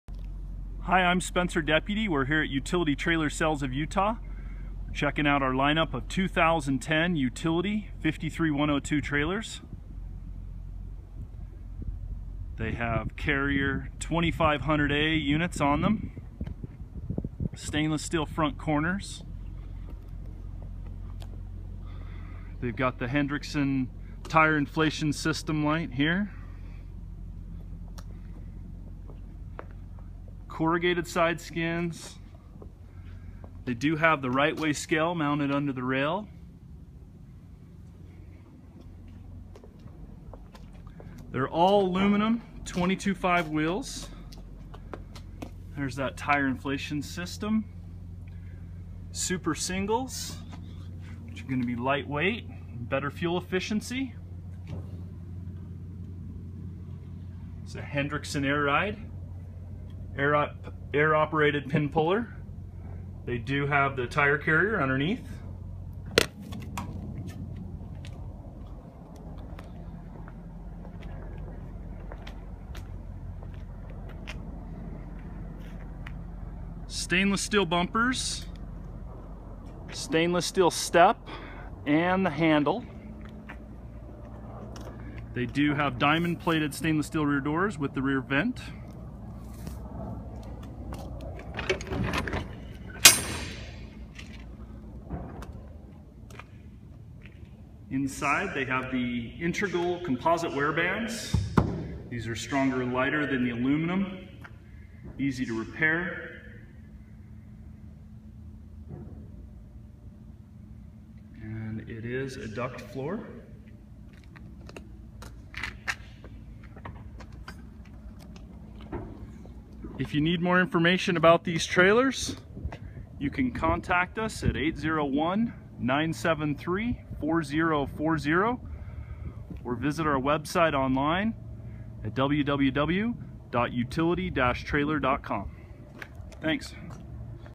2010 USED UTILITY REEFER
Walk around video of 2010 Utility 3000R 53'x102" Reefer with Carrier 2500A unit.  This one of many 2010 available in our yard here in Salt Lake City, Utah.